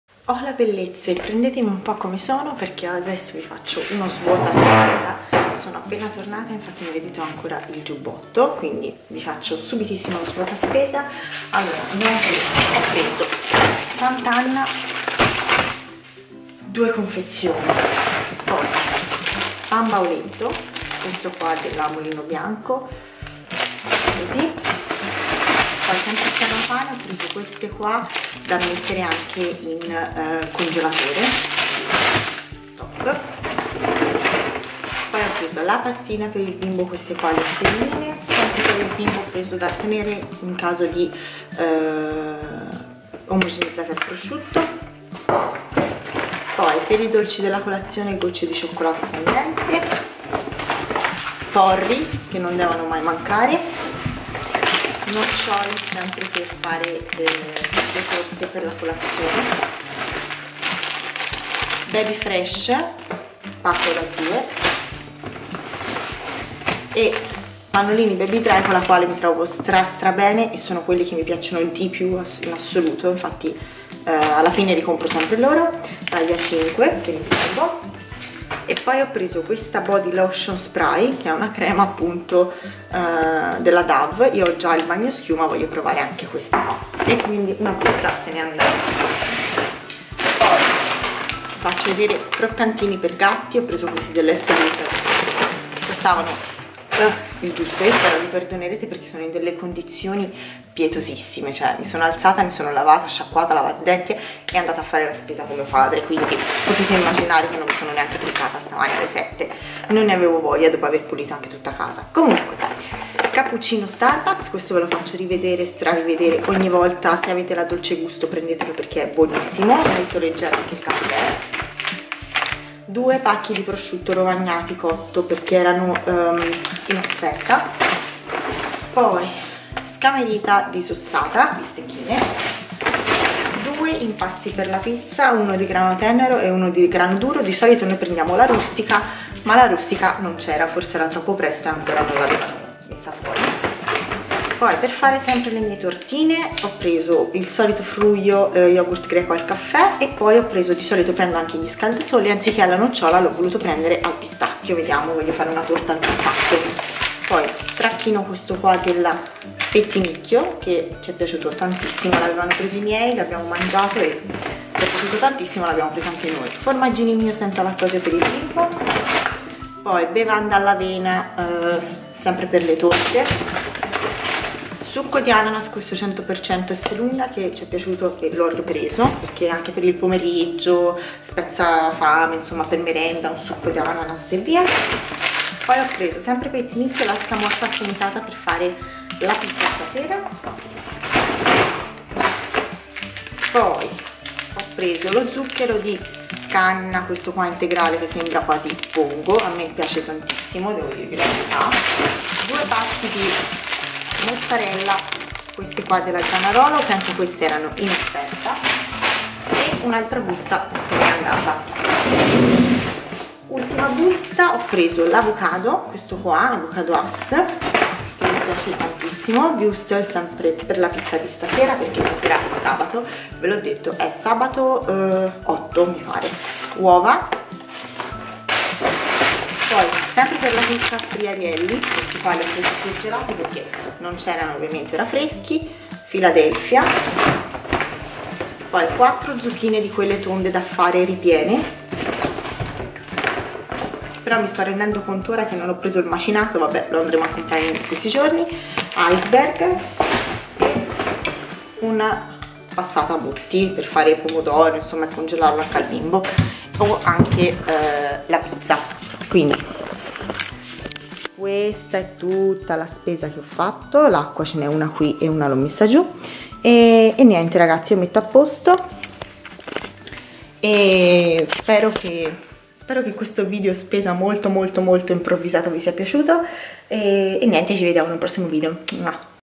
PRIMO SVUOTA SPESA ESSELUNGA!! | Lamiavitadamamma
Hola bellezzE ♡
Altra nuova tipologia di video che troverete ogni tanto sul canale.
Mi piace guardarli in primis da altre youtuber , anche solo per avere idee su cosa cucinare di diverso, ora che c'è anche il bimbo soprattutto.
Spero che vi piaccia, e vi possa esser di spunto per le vostre cene e pranzi futuri.
Come sempre vi aspetto qui sotto nei commenti,
al prossimo video!!

SEGUIMI ANCHE SU TIKTOK → _lamivitadamamma_